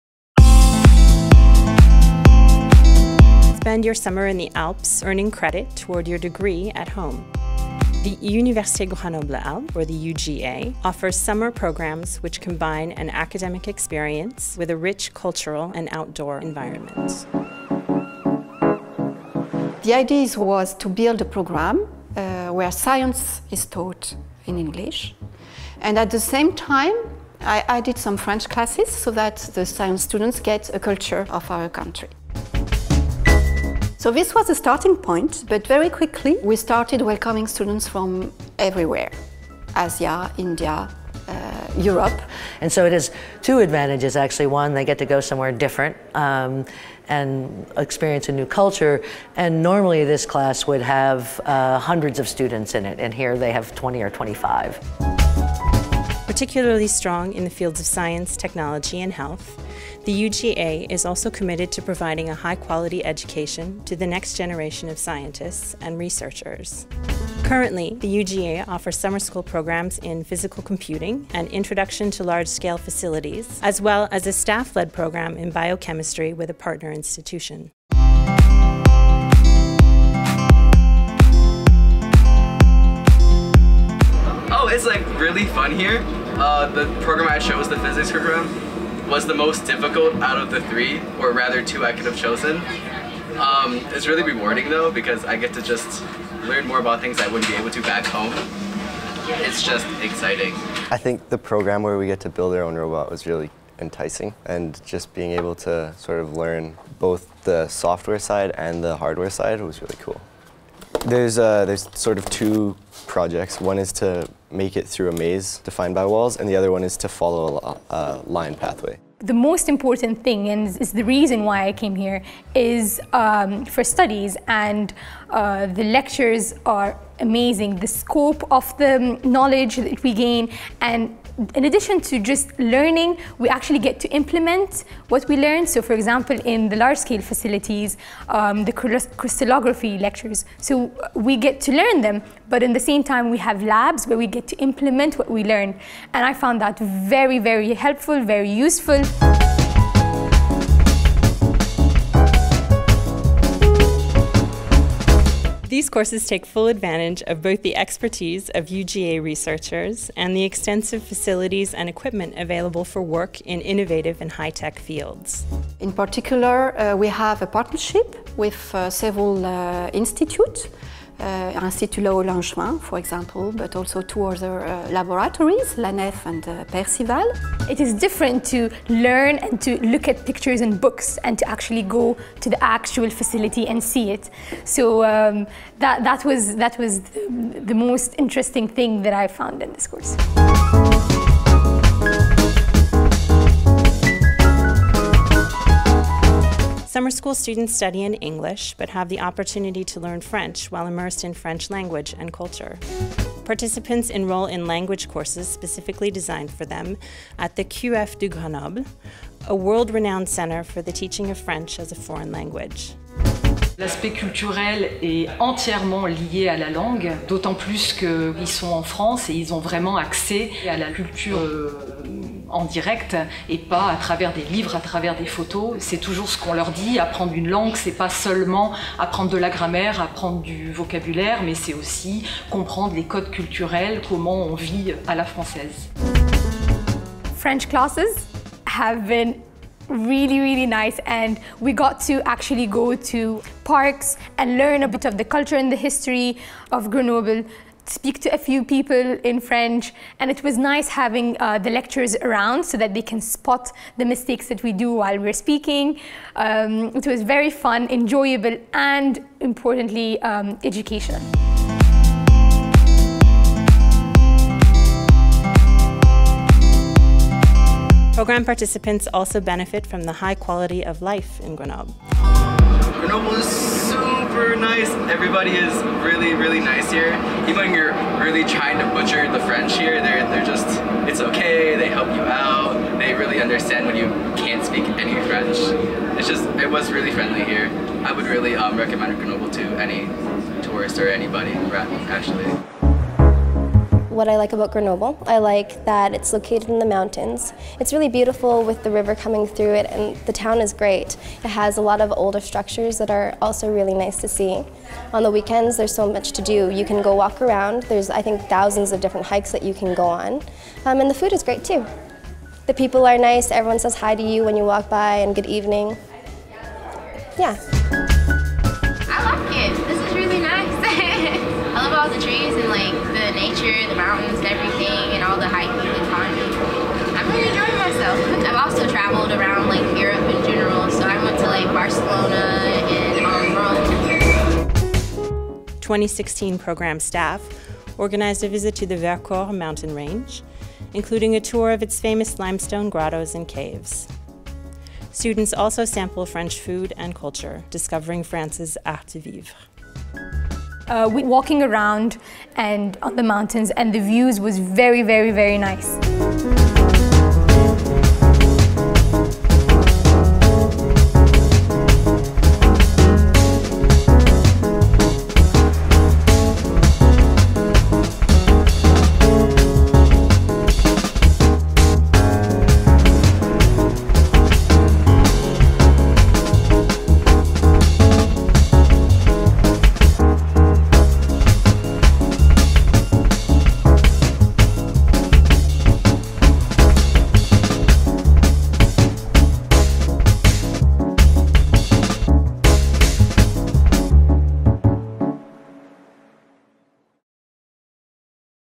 Discover UGA summer programs!
The Université Grenoble Alpes (UGA) offers short programs which combine an academic experience with a rich cultural and outdoor environment. Summer school students study in English, but have the opportunity to learn French while immersed in French language and culture. Some students provide feedback on the UGA summer program.